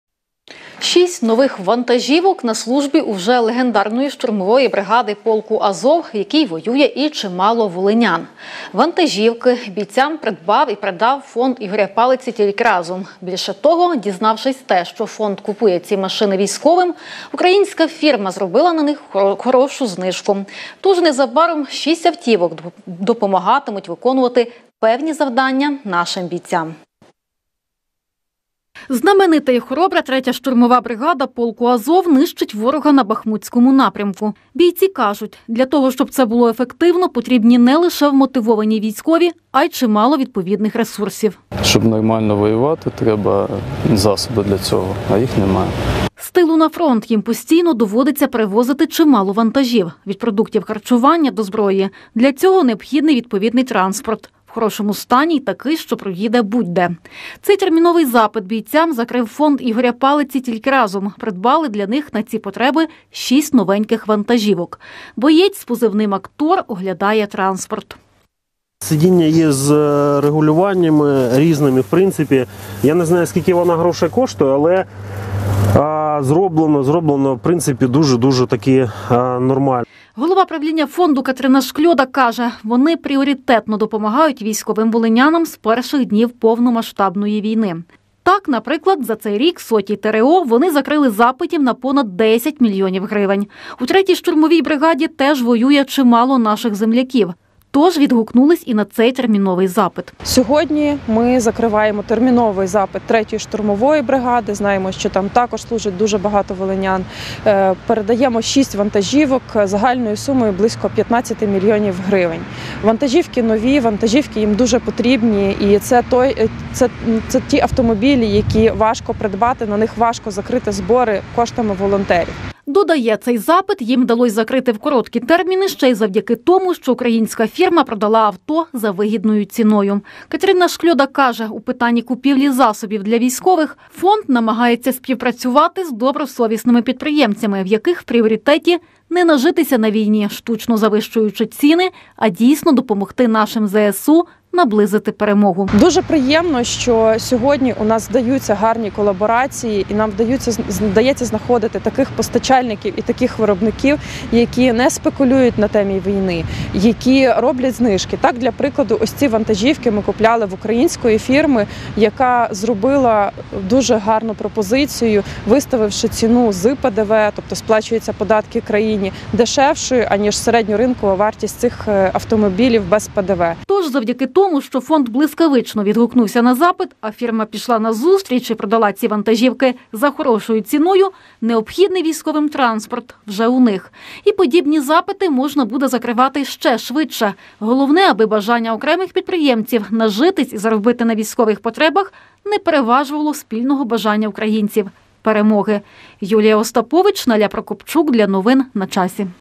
Третій штурмовій бригаді вручили ключі від шести нових вантажівок
Шість нових вантажівок - на службі у вже легендарної третьої штурмової бригади, в якій воює і чимало волинян. Транспорт  бійцям придбав і передав фонд Ігоря Палиці «Тільки Разом».  Більше того, дізнавшись те, що фонд купує ці машини військовим – українська фірма зробила на них хорошу знижку. Тож незабаром шість машин допомагатимуть  виконувати певні завдання нашим бійцям.